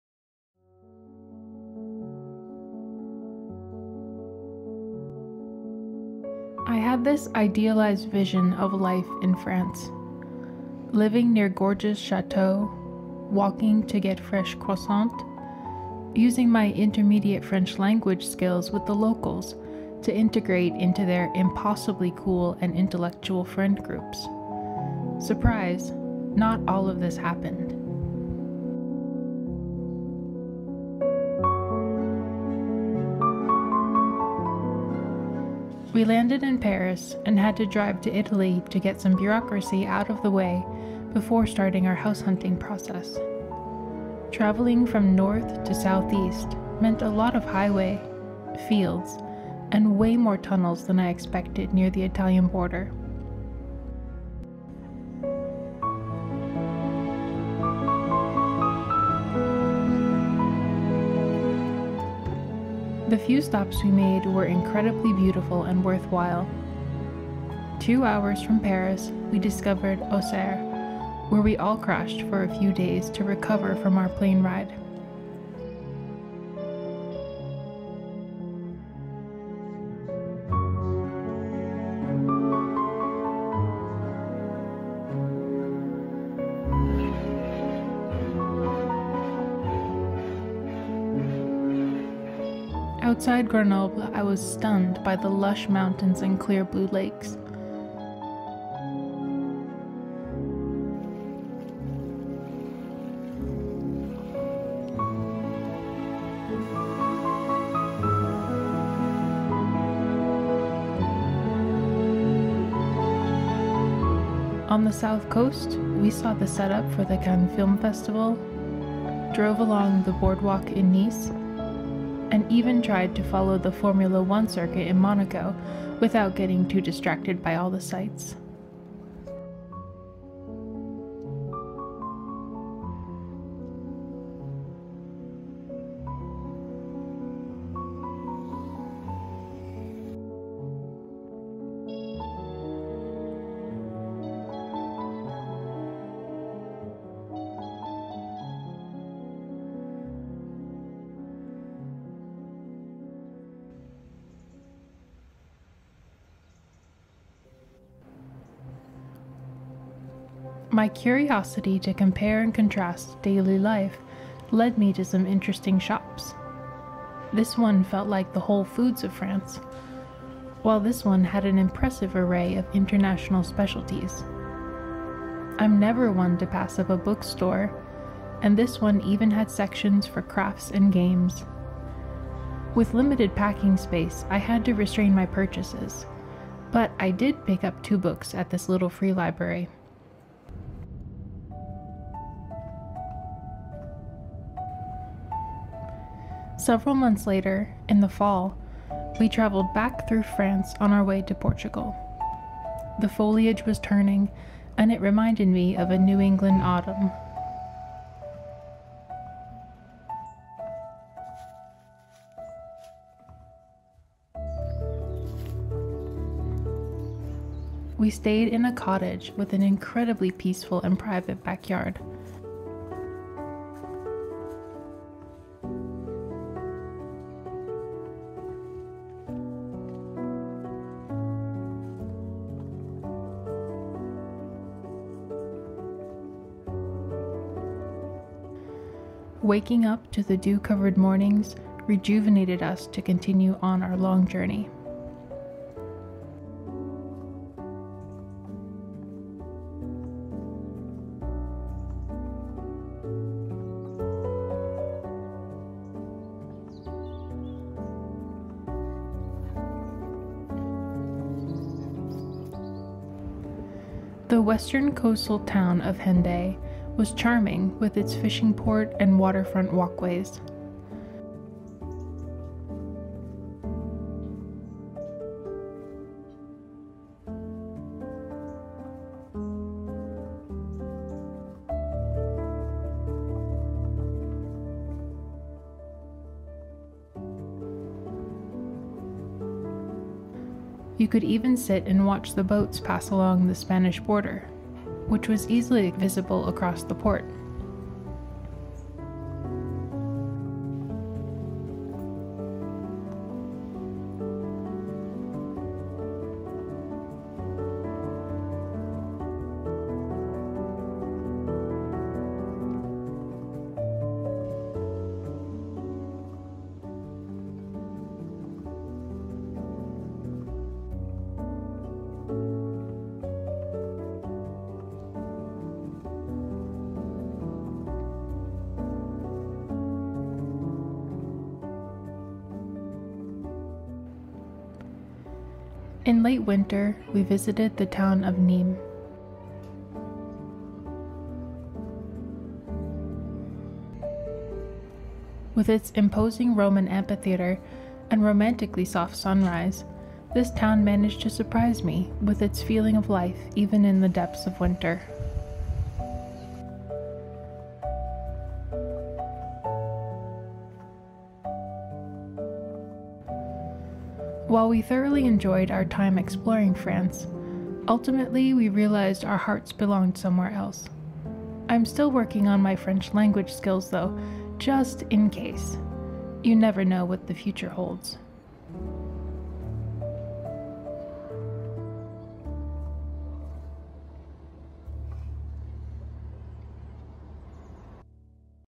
Traveling through France | Nice, Grenoble, Monaco
This video is about our travels in France so far, visiting different regions in summer, fall, and winter. These clips are from Auxerre, Dissay, Grenoble, Thyez, Nîmes, Soignolles-en-Brie, Nice, Hendaye, and Monaco.